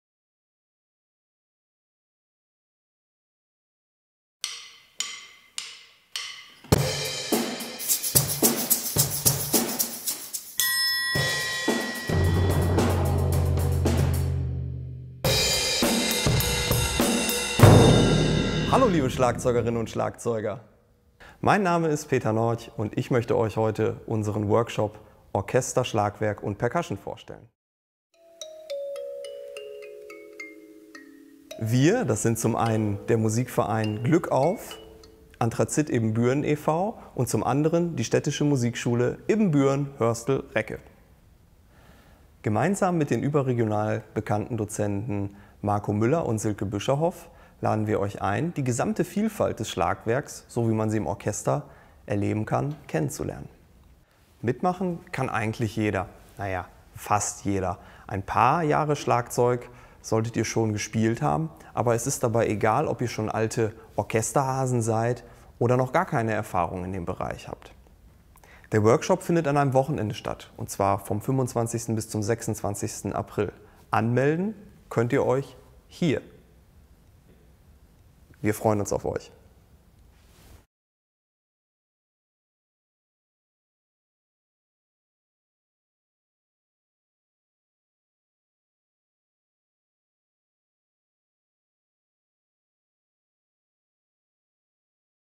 Workshop - Orchesterschlagwerk und Percussion
25.-26. April in der Musikschule Ibbenbüren

Dozenten:
Marco Müller - Musikkorps der Bundeswehr
Silke Büscherhoff - Musikschule Ibbenbüren